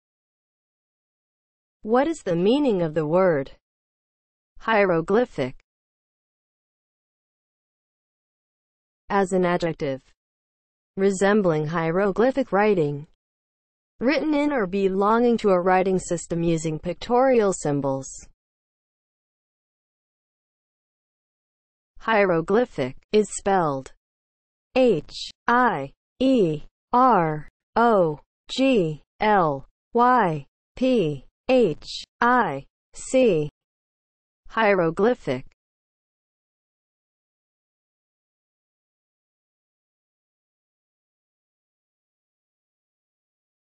What is the meaning of the word HIEROGLYPHIC?
Definition and spelling of the word HIEROGLYPHIC.

(00:00) Intro
(00:08) As an adjective
(00:20) Spelling

As an adjective (00:08):
• resembling hieroglyphic writing
• written in or belonging to a writing system using pictorial symbols